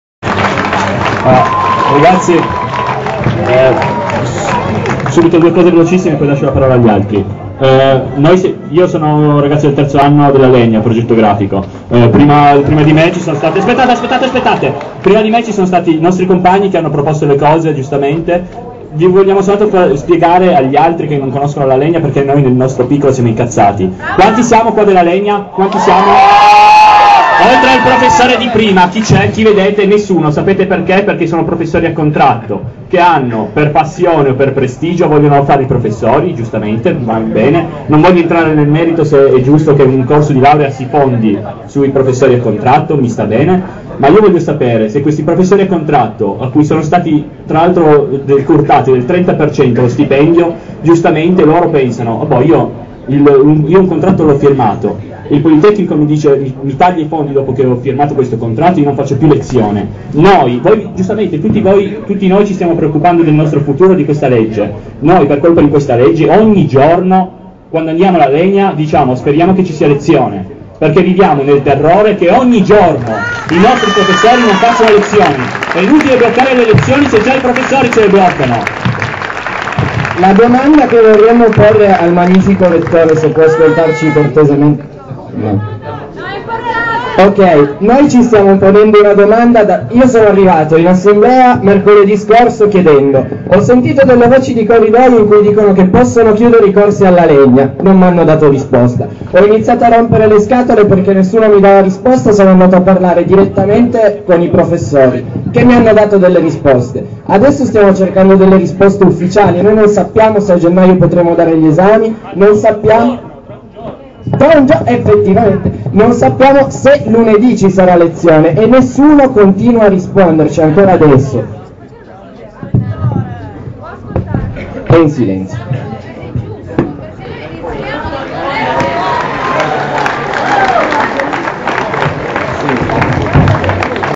PoliTo - legge 133 [6]
Politecnico di Torino - manifestazione contro legge 133 23/10/08
Interventi dei ragazzi